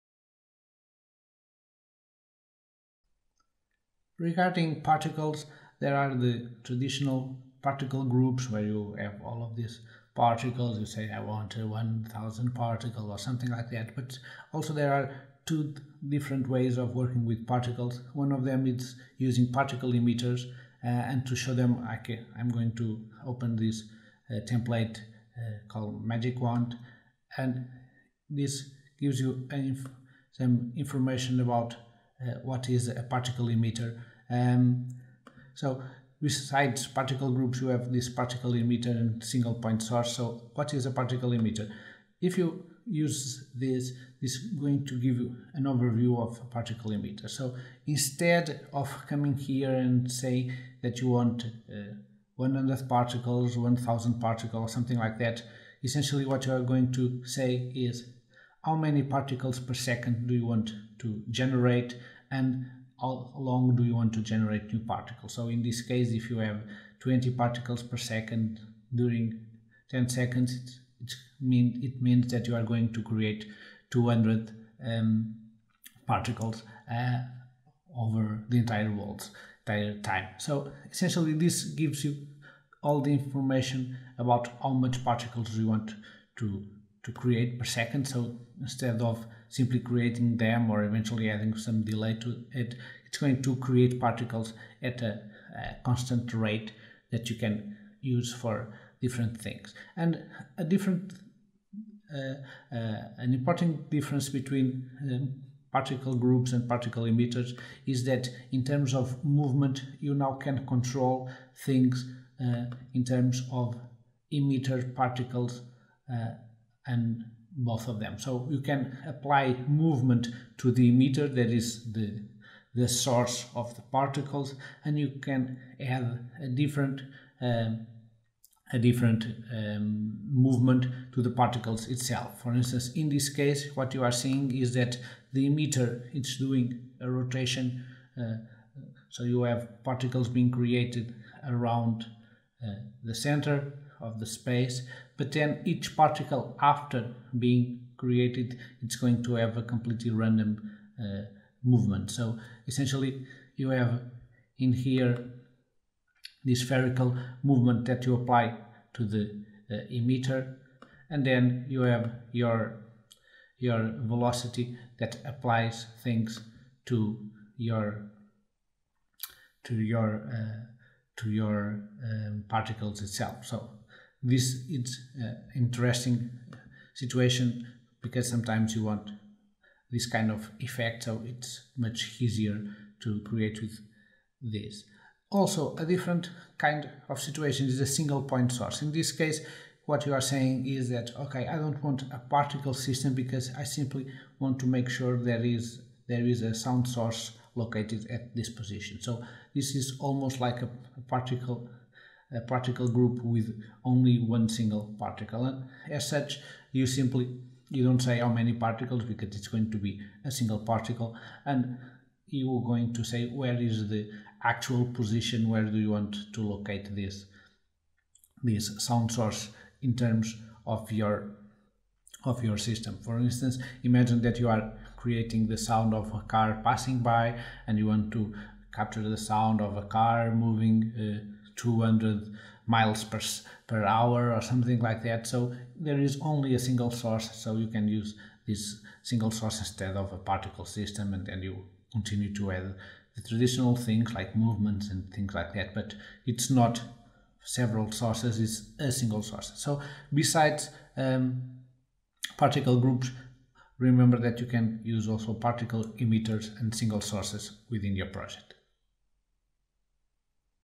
Other Particles - Sound Particles 1 Video Tutorial (11/14)
Particle emitters and single sound sources inside Sound Particles - a particle system software for audio.